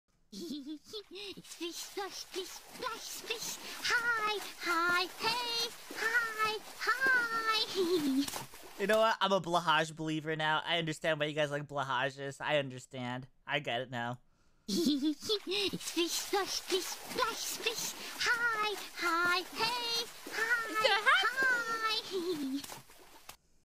Nano now into Blahaj
This is a clip from Nano Nano's Twitch stream entitled WHAT IS SHE DOING? which originally streamed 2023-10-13.

Nano Nano retains all rights to the stream content.